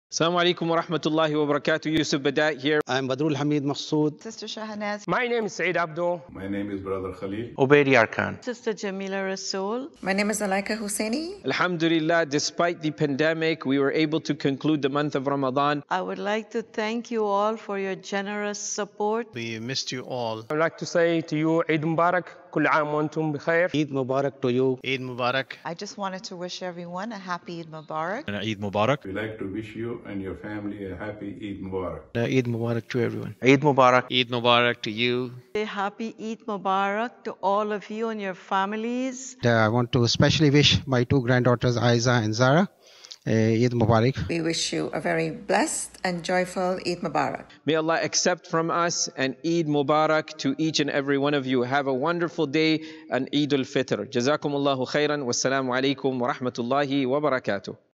Eid Mubarak FROM IFT
Staff and volunteers from the Islamic Foundation wish you all an Eid Mubarak!